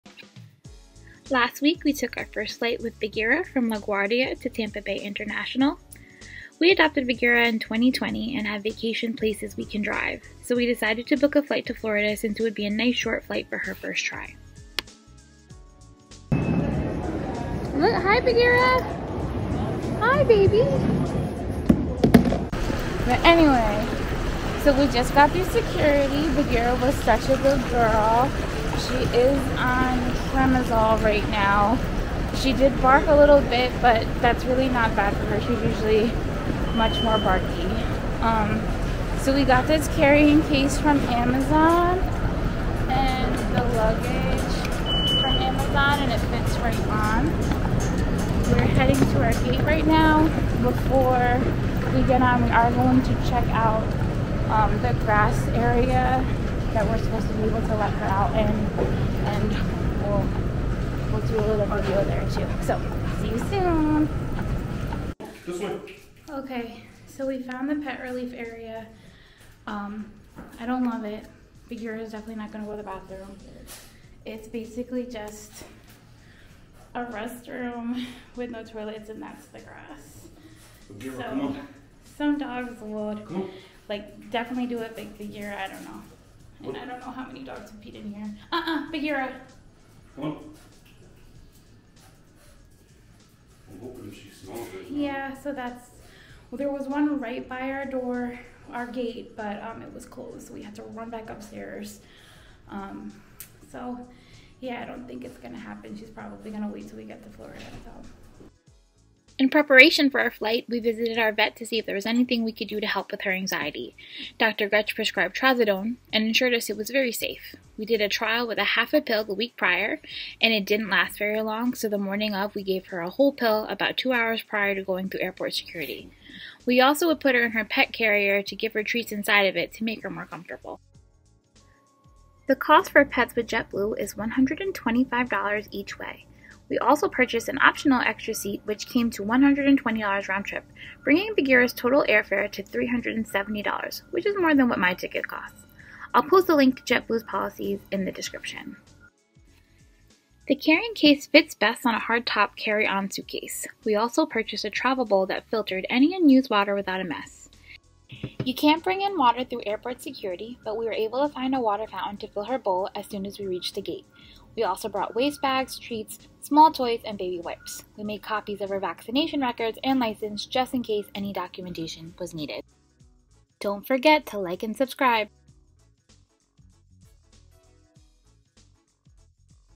Flying Jetblue with My French Bulldog
Wonder what it's like to fly with your frenchie? Join us on our first ever flight with Bagheera on Jetblue!

Links!
Airline Approved Pet Carrier:

Carry On Luggage:

Filtered Dog Travel Bowl: